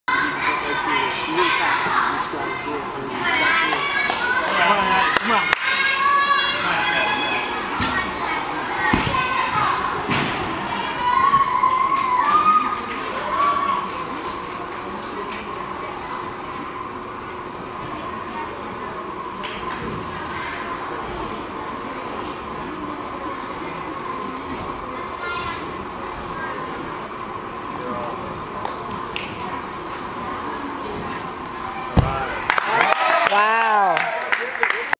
Ashley Ables 2nd Bars Gymcats Invite Wildfire Gymnast Level 3
2nd Place Bars 9.550. All Around Champion 38.275.